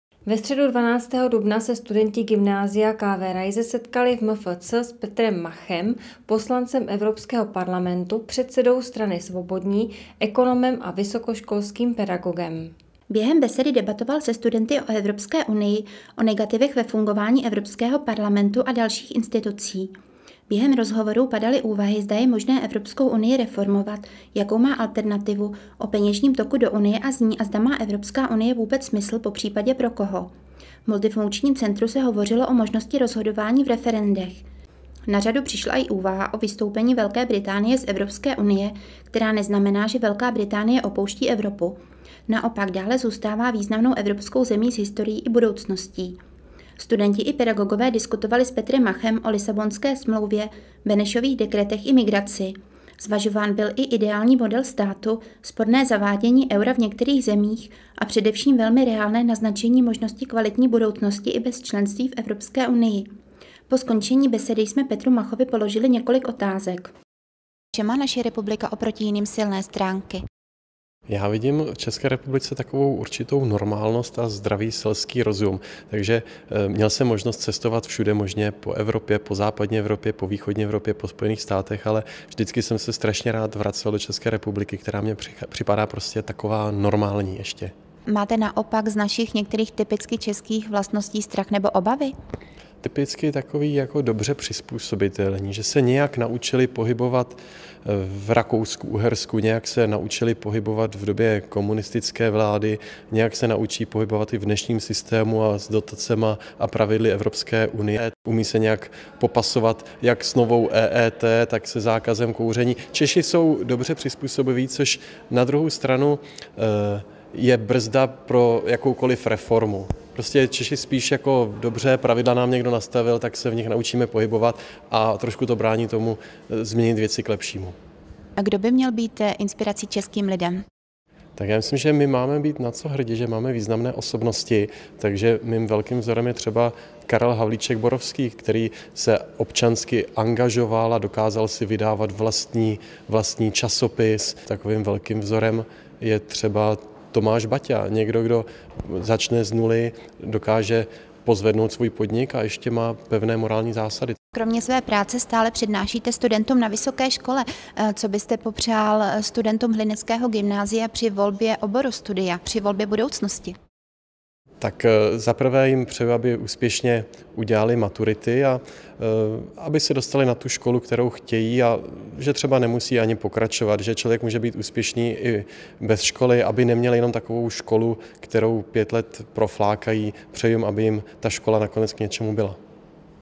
S Petrem Machem nejen o Evropské unii
Ve středu 12. dubna se studenti Gymnázia K. V. Raise setkali v MFC s Petrem Machem, poslancem Evropského parlamentu, předsedou strany Svobodní, ekonomem a vysokoškolským pedagogem.